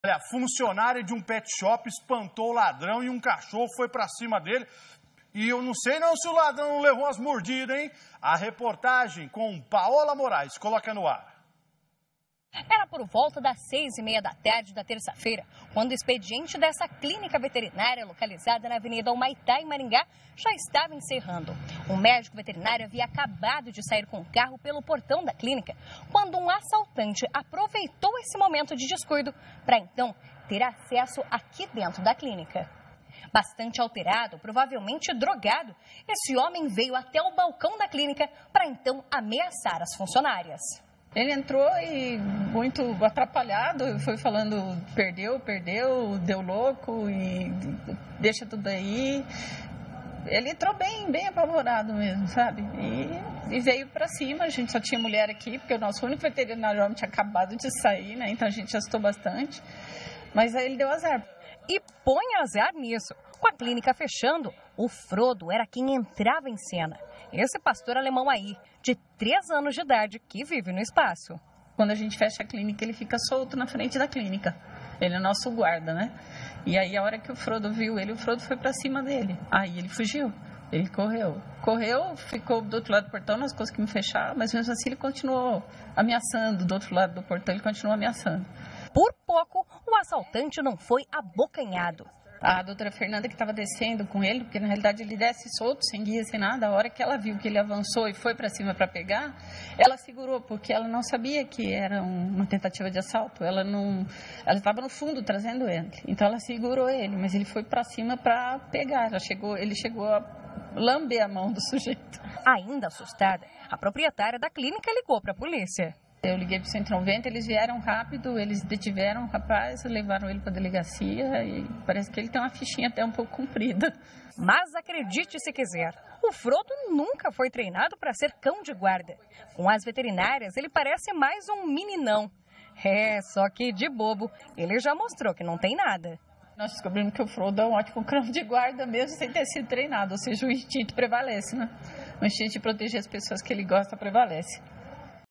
Cachorro coloca ladrão para correr em clinica veterinária
O expediente da clinica estava encerrando quando um homem anunciou o assalto.  O cachorro, da raça Pastor Alemão, cuida do local quando está fechado e avançou em cima do suspeito, que correu para fora do local.